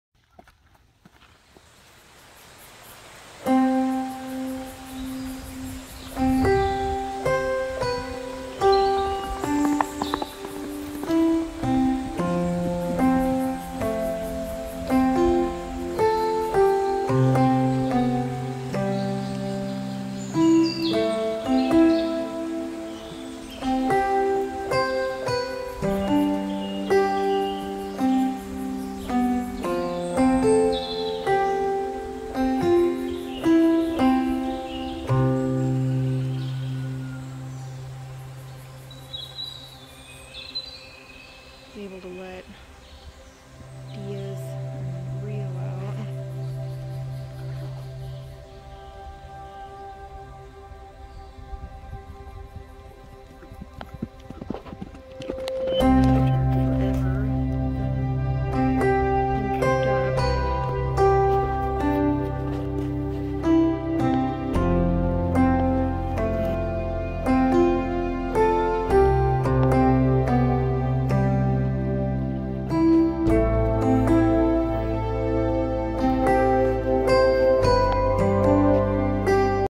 Spanish Barb - Sulphur Colt Shows Off
Diaz was born June 30, 2019 by Dragon's Sulphur Breath x Tiger Lily. His bloodlines are pure Sulphur strain.